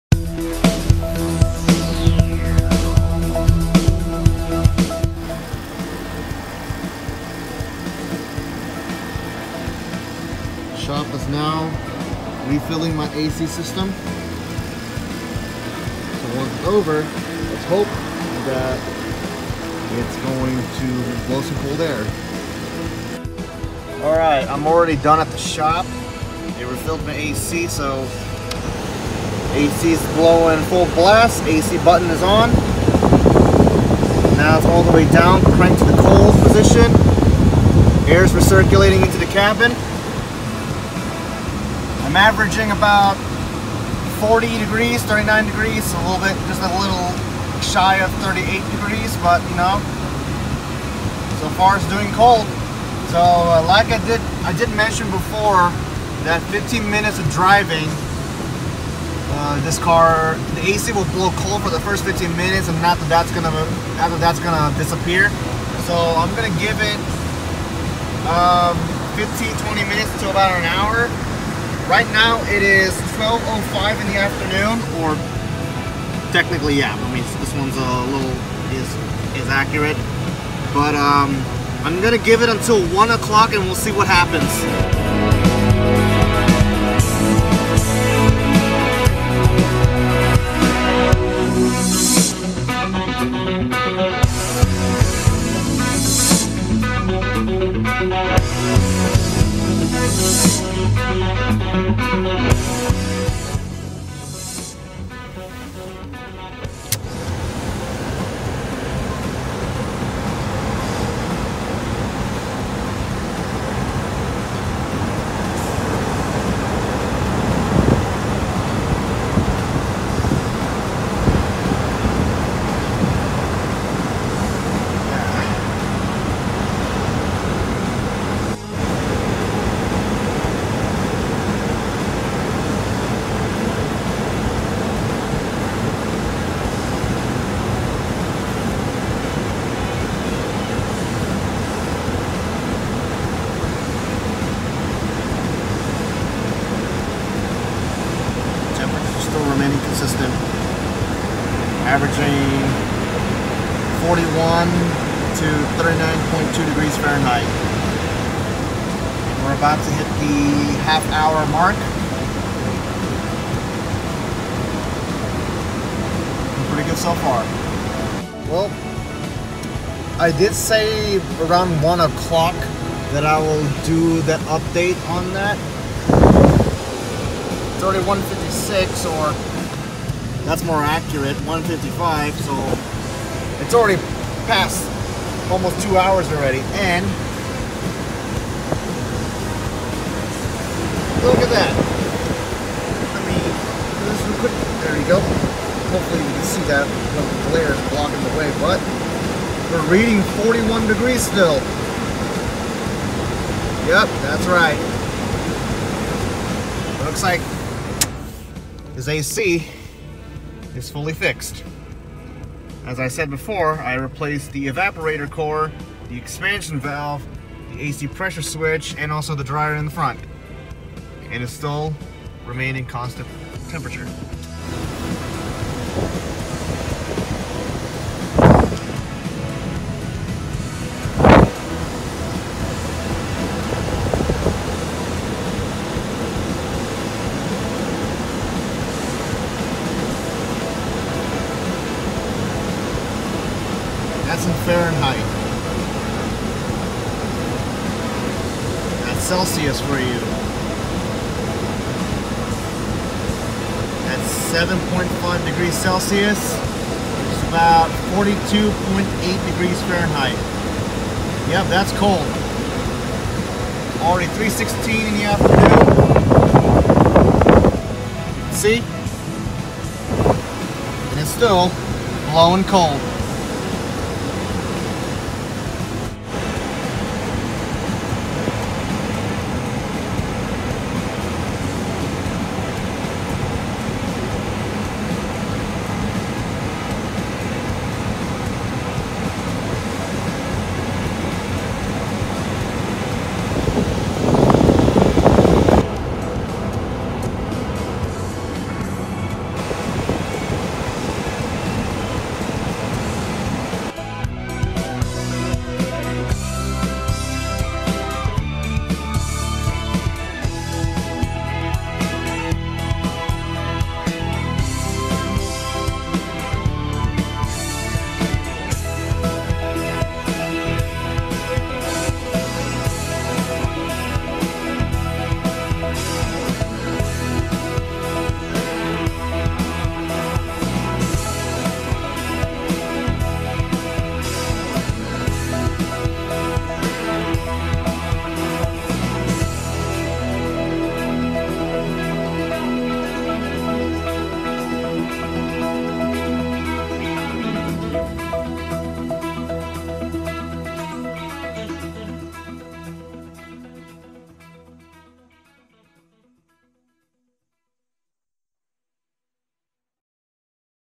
2nd Gen Toyota 4Runner AC Evaporator Core Replacement PART 2 (Refilling AC System and Test)
This is just a follow up on my previous video of me replacing the AC evaporator core on my 2nd gen Toyota 4Runner